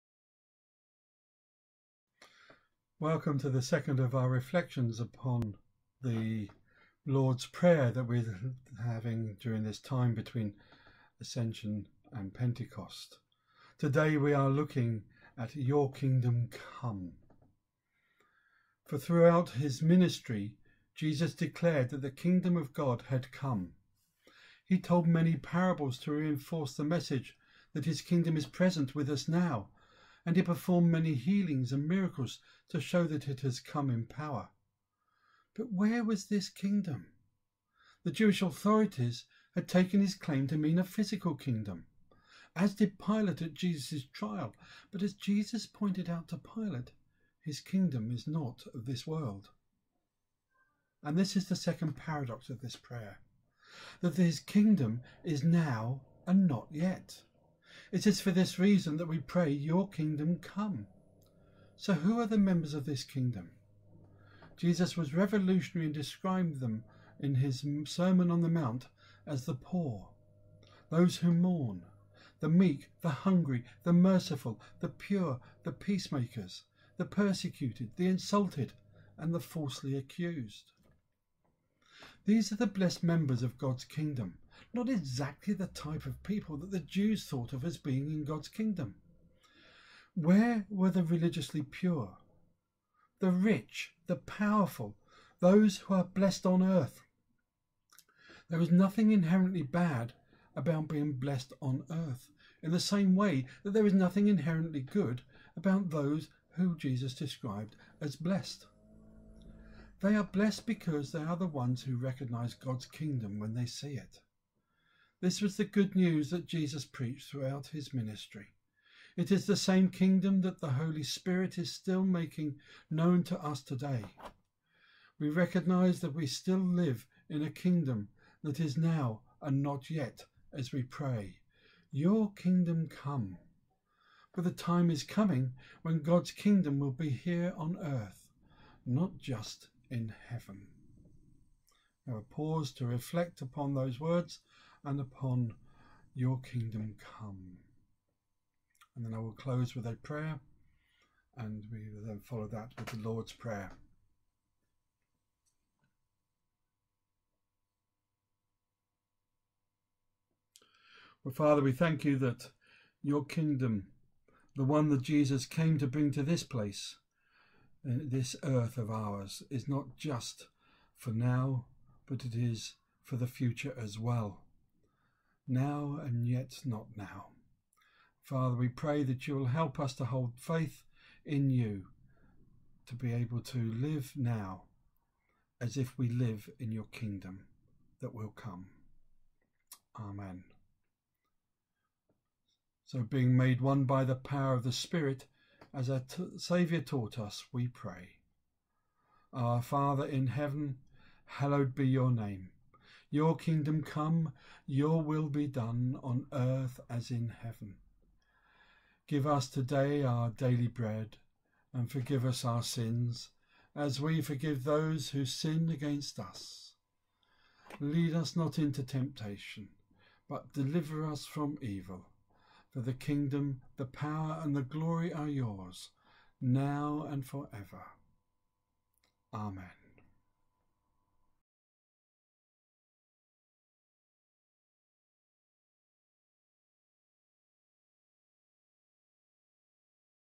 Thy Kingdom Come #2 'Your will be done'
Daily reflection in The Lord's Prayer from Ascension to Pentecost 2020.